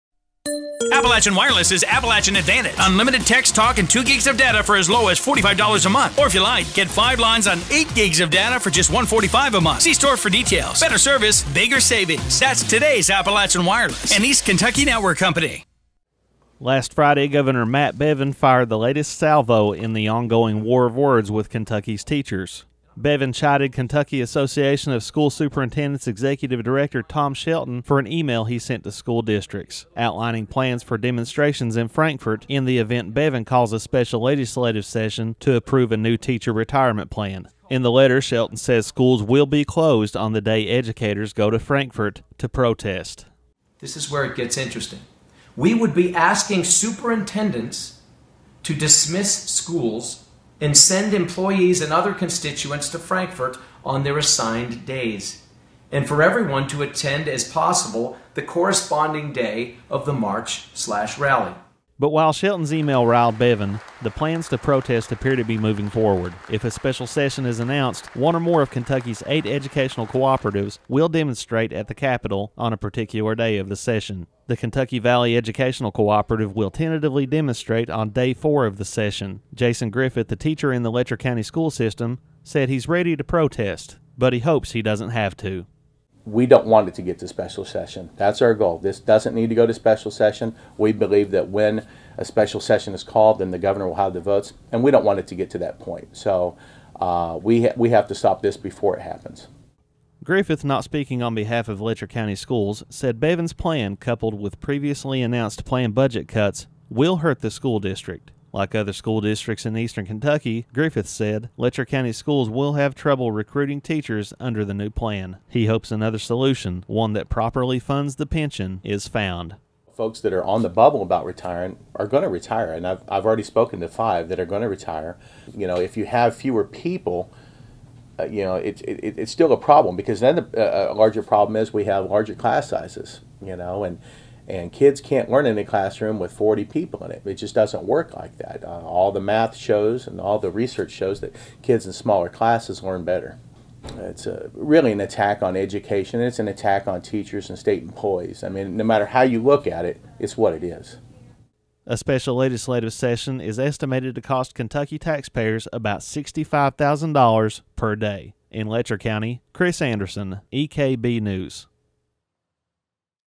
Educators set to protest if special legislative session called
The rift between Kentucky’s educators and the governor over pension reform continues to grow. And if the governor makes good on his plans to call a special legislative session to pass a new retirement plan, teachers across the Commonwealth plan to descend upon Frankfort to let their voices be heard. EKB News Reporter Chris Anderson spoke to one such educator. He has this report.